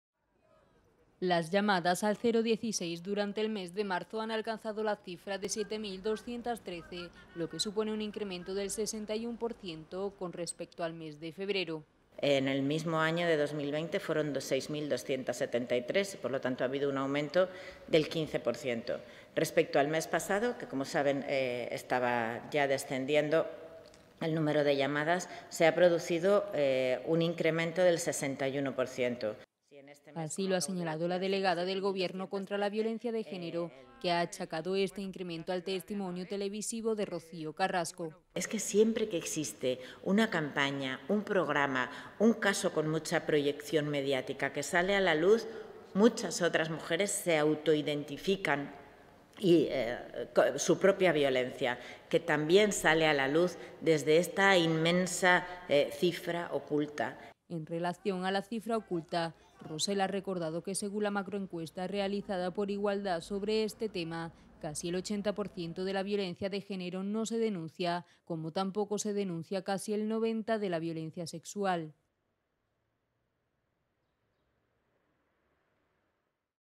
Aumentan las llamadas al 016 en marzo tras el testimonio de Rocío Carrasco
Las llamadas al 016 durante el mes de marzo han alcanzado la cifra de 7.213, lo que supone un crecimiento del 61%, con respecto al mes de febrero. Así lo ha señalado la Delegada de Gobierno contra la Violencia de Género, que ha achacado este incremento al testimonio televisivo de Rocío Carrasco.
(Fuente: Europa Press, Ministerio de Igualdad)